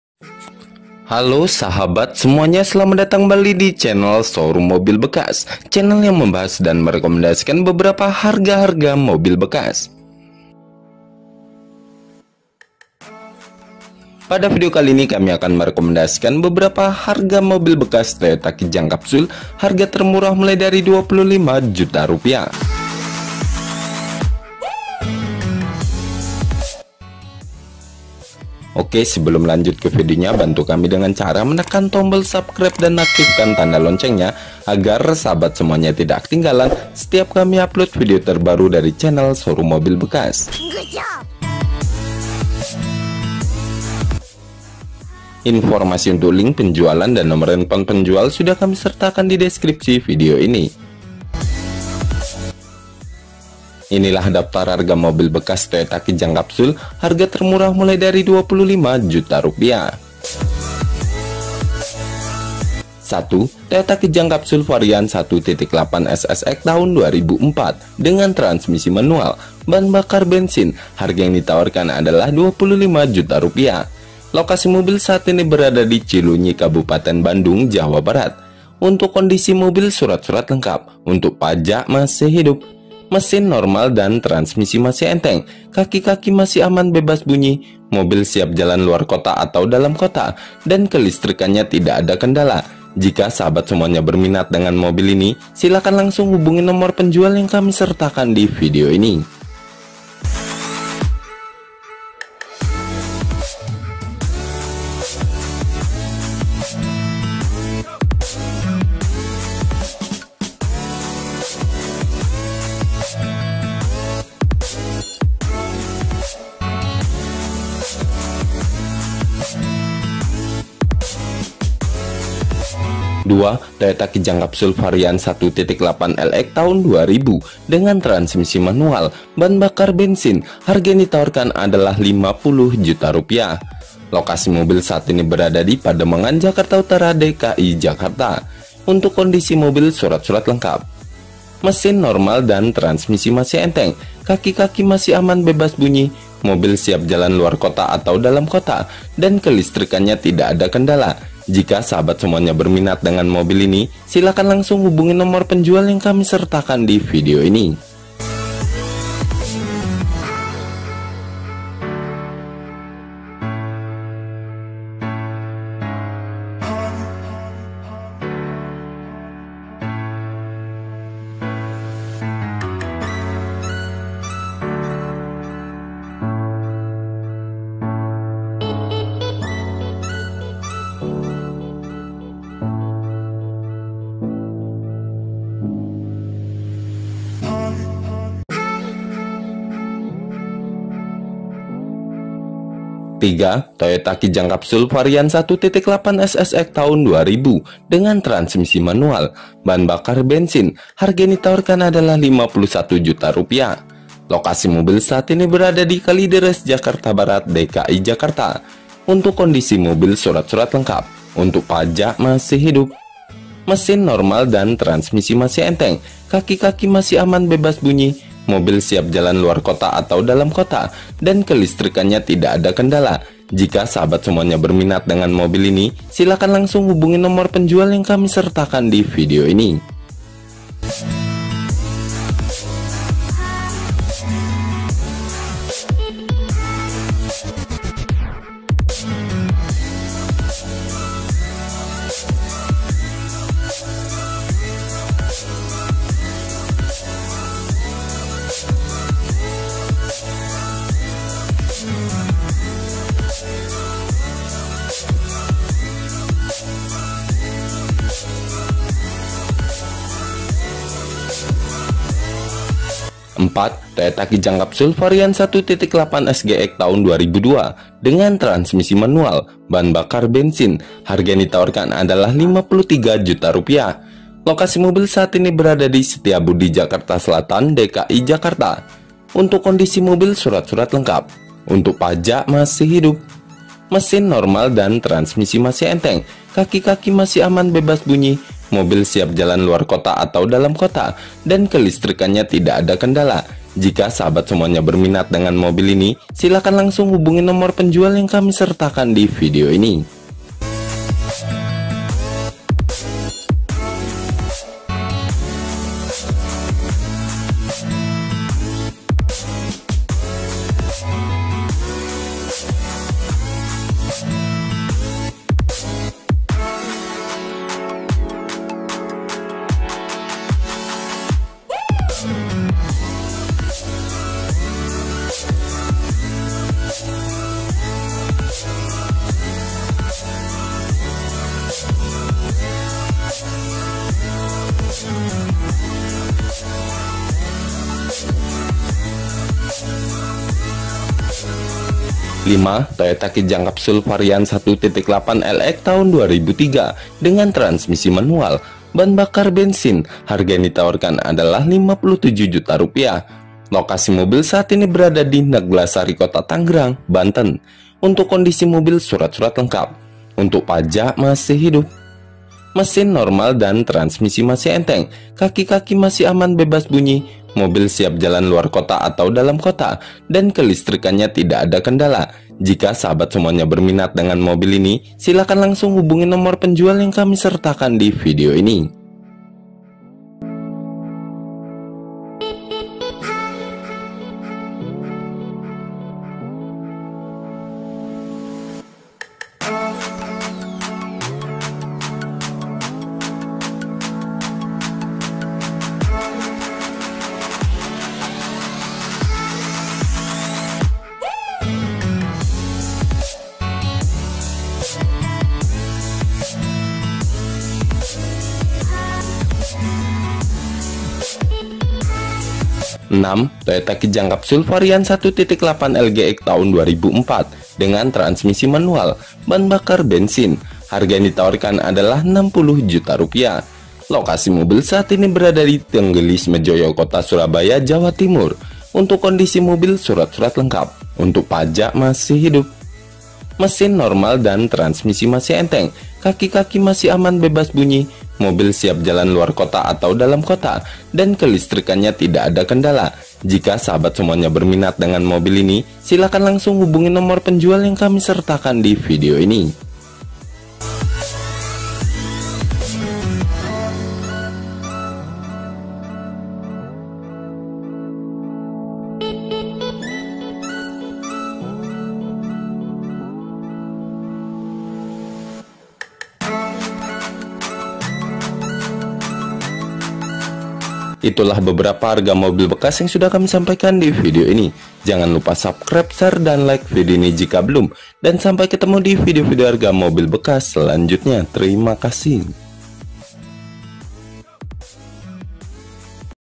HARGA MOBIL BEKAS KIJANG KAPSUL TAHUN 2004 HANYA 25 JUTA
HARGA MOBIL BEKAS TOYOTA KIJANG KAPSUL

1. TOYOTA KIJANG KAPSUL 1.8 SSX TAHUN 2004, HARGA 25 JUTA

2. TOYOTA KIJANG KAPSUL 1.8 LX TAHUN 2000, HARGA 50 JUTA

3. TOYOTA KIJANG KAPSUL 1.8 SSX TAHUN 2000, HARGA 51 JUTA

4. TOYOTA KIJANG KAPSUL 1.8 SGX TAHUN 2002, HARGA 53 JUTA

5. TOYOTA KIJANG KAPSUL 1.8 LX TAHUN 2003, HARGA 57 JUTA

6. TOYOTA KIJANG KAPSUL 1.8 LGX TAHUN 2004, HARGA 60 JUTA

TERIMA KASIH